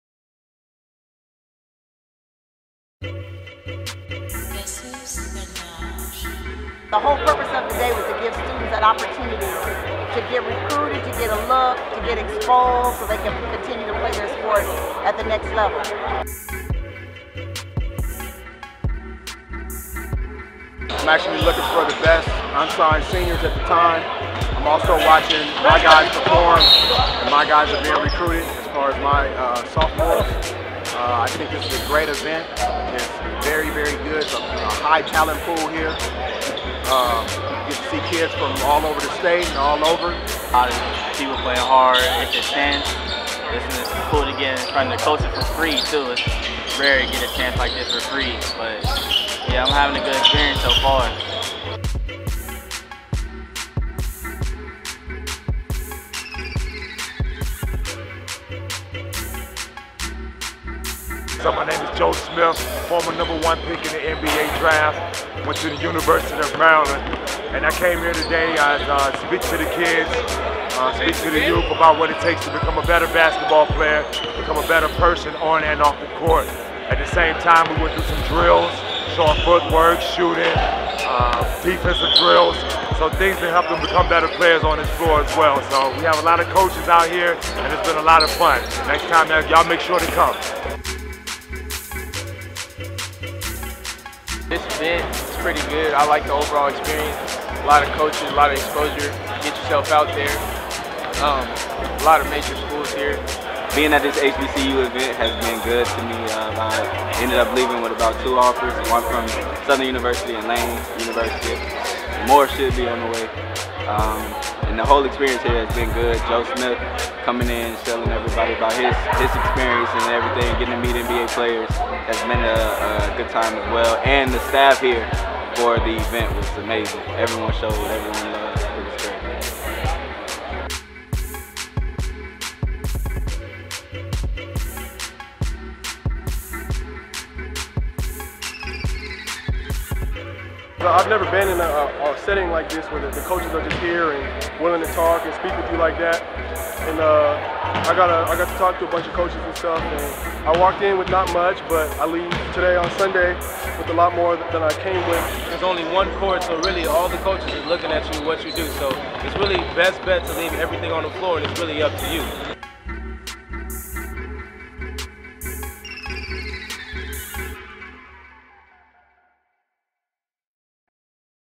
HBCU Basketball Showcase 2018
The inaugural HBCU Basketball Showcase at Lynwood High School hosted 21 College Coaches & over 200 players resulting in 57 Athletic scholarships!